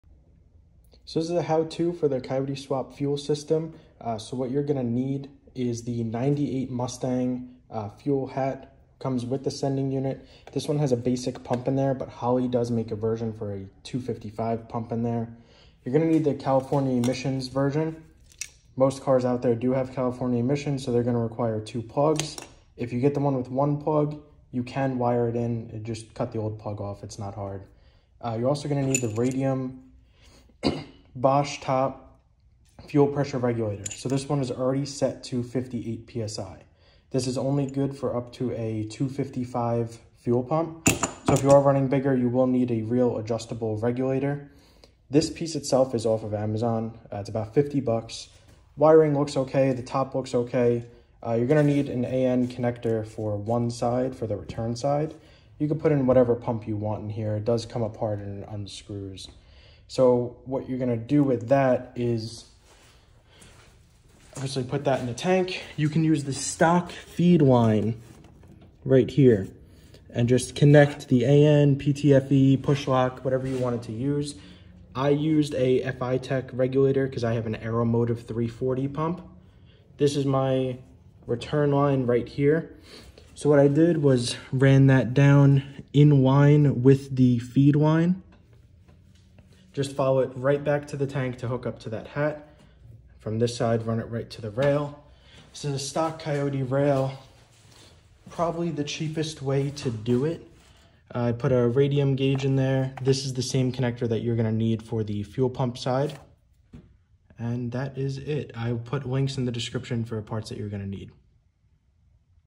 HOW TO: Coyote swap fuel system newedge or sn95 mustang 94-04
Sn95/newedge coyote swap fuel system. Simple and cost effective. Use a holley tu227hp or a ti automotive unit as they come with a 225lph pump.

You can use AN lines, PTFE or pushlok.
 Here is the linked parts below!

fuel hat

Aeromotive Fuel pump to install

PTFE fuel lines

Fuel pressure regulator